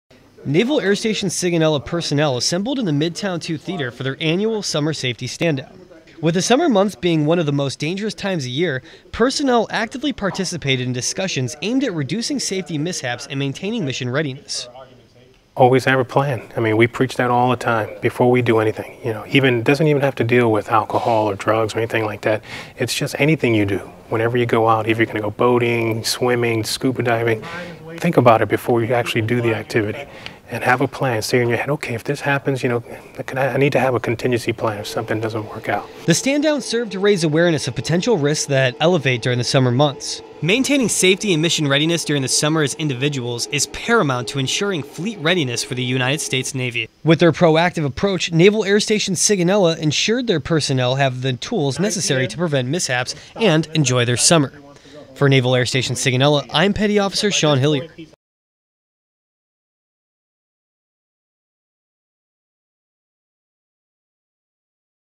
NAS Sigonella hosts Summer Safety Standdown
Sailors aboard Naval Air Station Sigonella learn how to be as safe as possible during their summer months.  Petty Officer Sean Hillier has more.